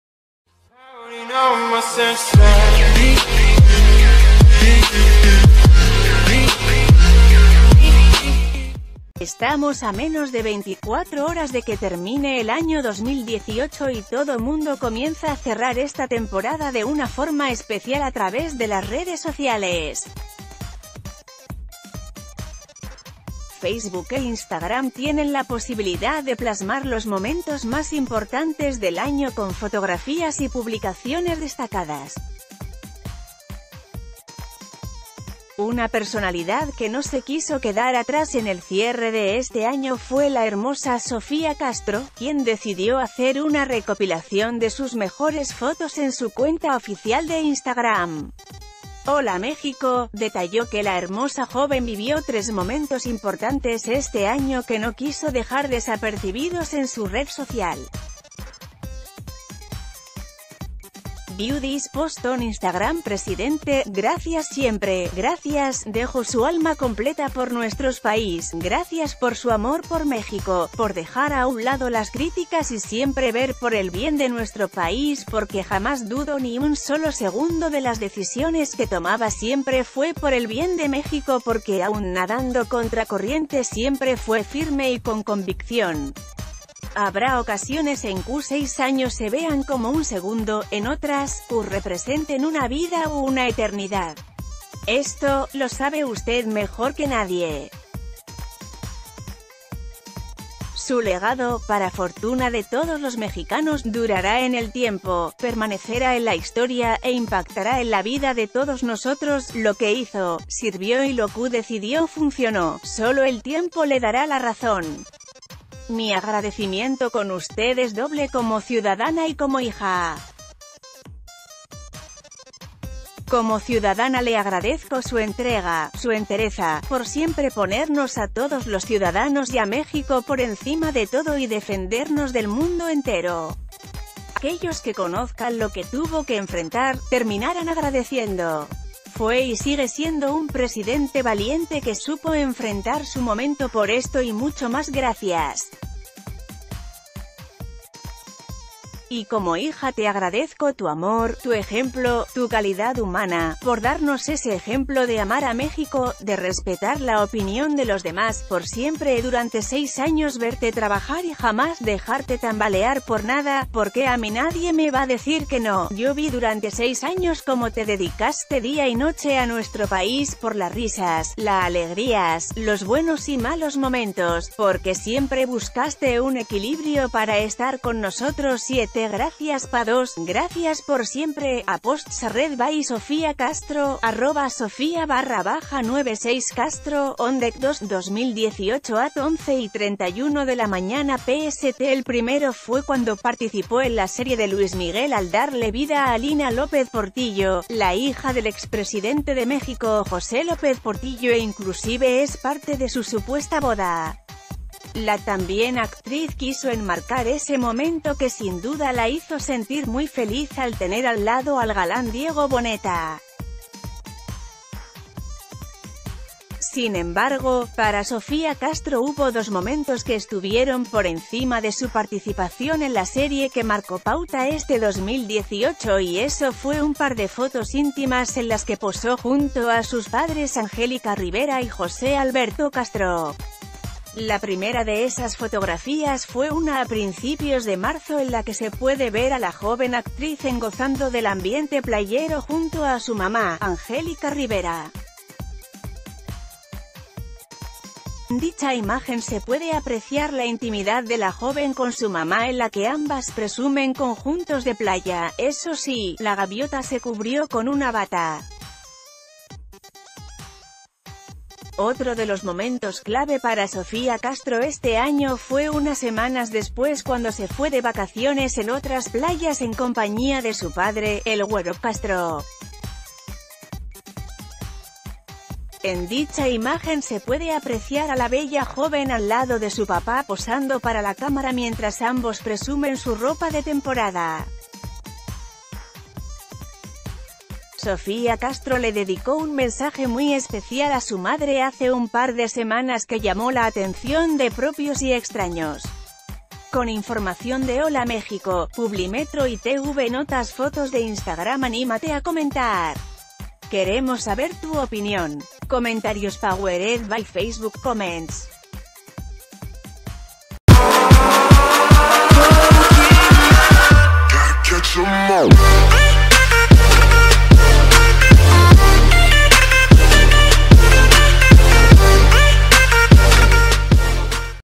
Sofía Castro concluye el año compartiendo fotos íntimas con sus padres
Facebook e Instagram tienen la posibilidad de plasmar los momentos más importantes del año con fotografías y publicaciones destacadas. Una person...